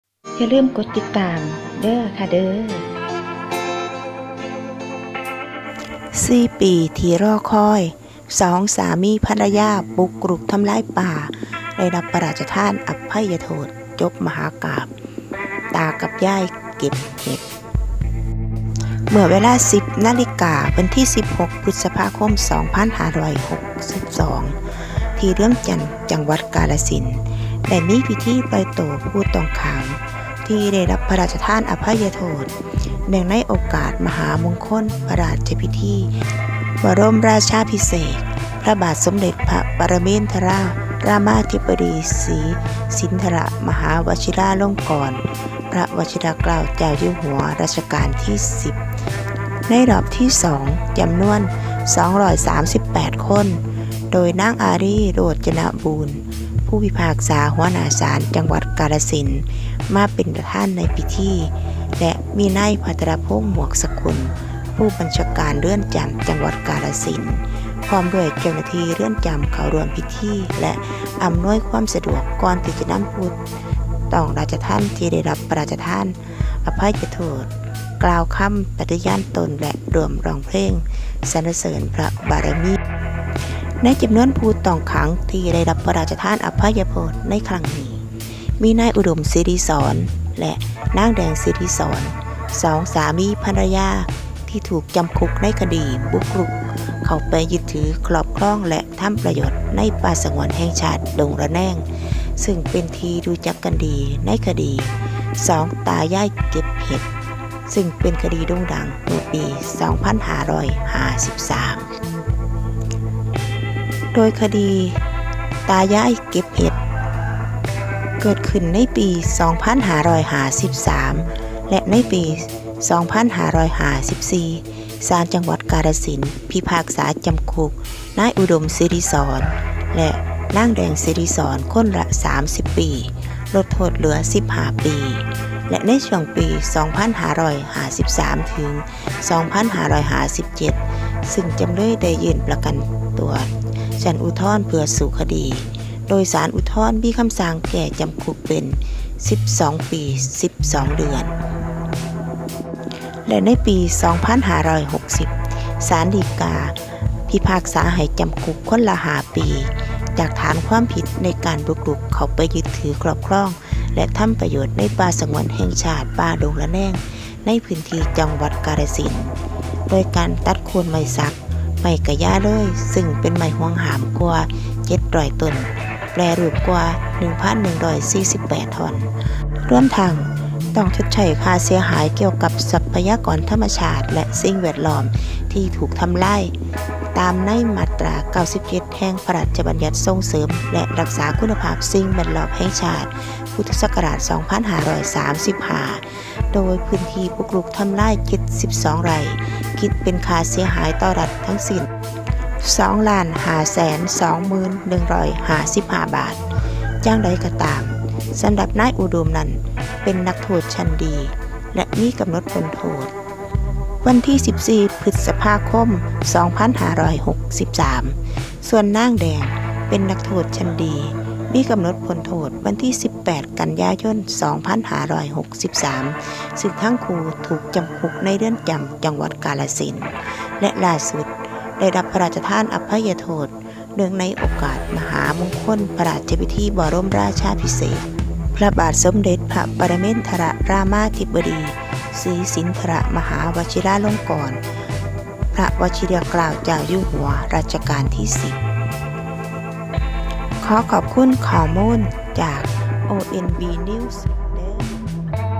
ตายาย (ไม่ได้)เก็บเห็ด ได้รับพระราชทานอภัยโทษ
จากคดีบุกรุกป่าสงวนแห่งชาติ ป่าดงระแนง จ.กาฬสินธุ์ เหตุเกิดเมื่อปี 2553  แต่ถูกนำมากระทบกระเทียบกับกระบวนการยุติธรรมของไทย ในชื่อ "ตายายเก็บเห็ด" และศาลฎีกาได้ตัดสินจำคุกเมื่อปี 2560
16 พฤษภาคม 2562 สองสามีภรรยา ได้รับพระราชทานอภัยโทษ คืนสู่อิสระภาพ

Cr.ONB news.
     MGR. online